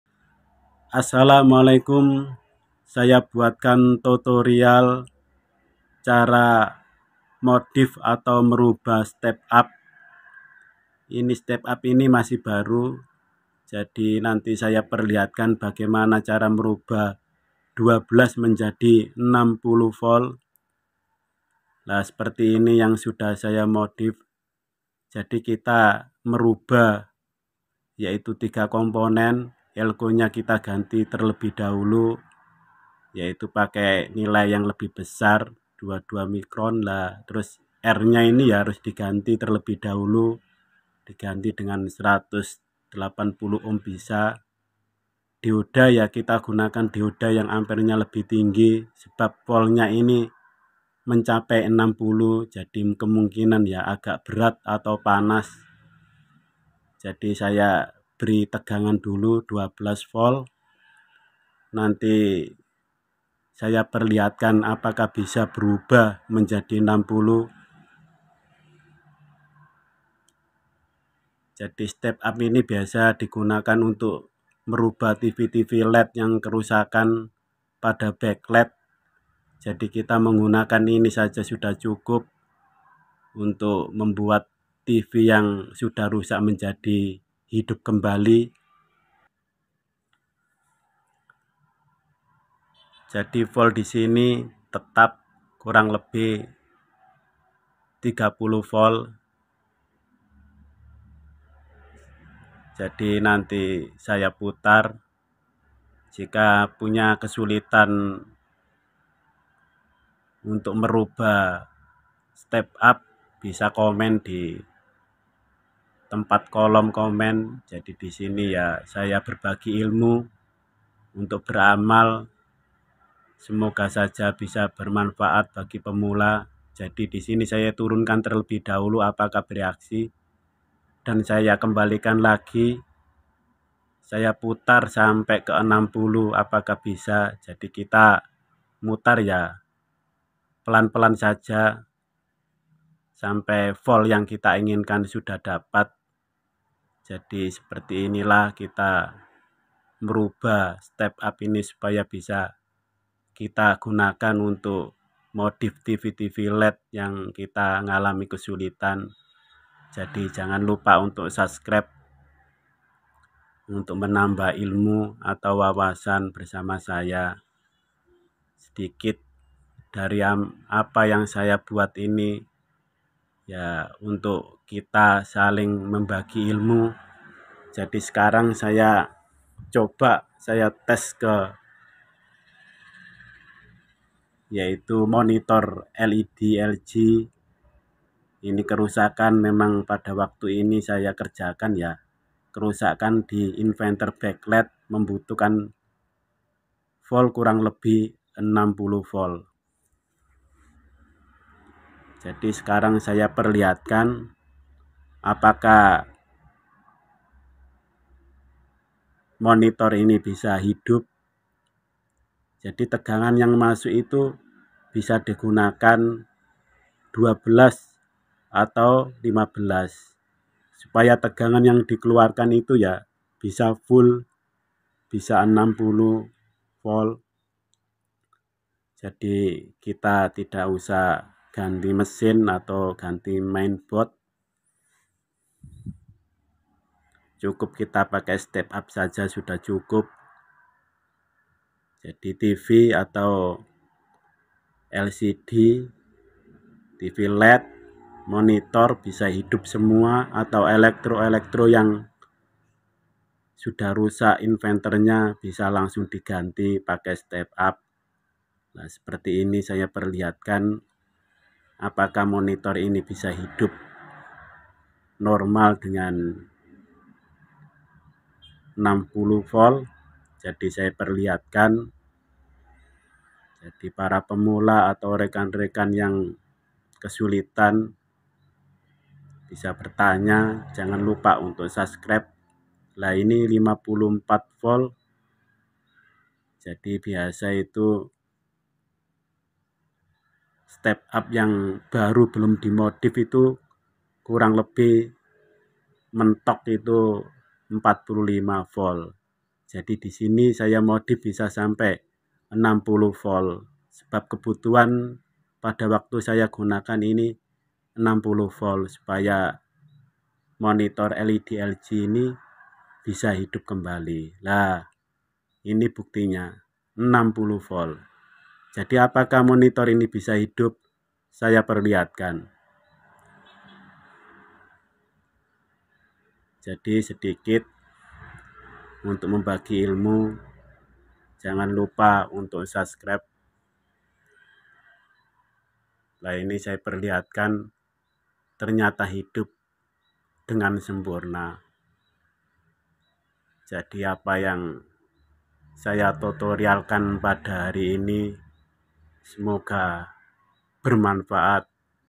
Cara pasang step up untuk modif tv led 12vol sampai 61.5vol full semua elektronik
Cara pasang step up untuk modif tv led 12vol sampai 61.5vol full semua elektronik dc conventer bisa digunakan untuk modif semua elektronik penganti backlight tv / lampu led /bisa di gunakan untuk tegangan vol ic yang hilang. jika belum mengerti bisa bertanya . semoga video saya bermanfaat jangan lupa subscribe supaya tdk ketinggalan dengan video saya sekian, terimakasih